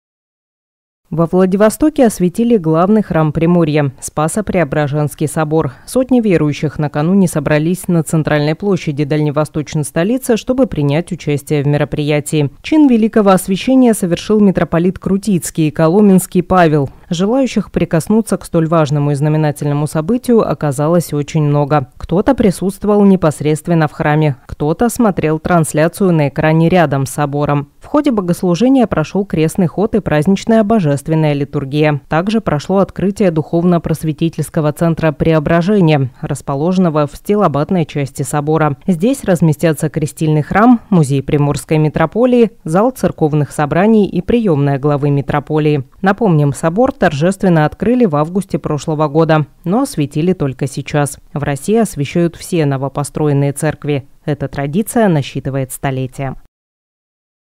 Во Владивостоке прошло освящение Спасо-Преображенского собора.
Во Владивостоке освятили главный храм Приморья – Спасо-Преображенский собор. Сотни верующих накануне собрались на центральной площади дальневосточной столицы, чтобы принять участие в мероприятии. Чин Великого освящения совершил митрополит Крутицкий и Коломенский Павел.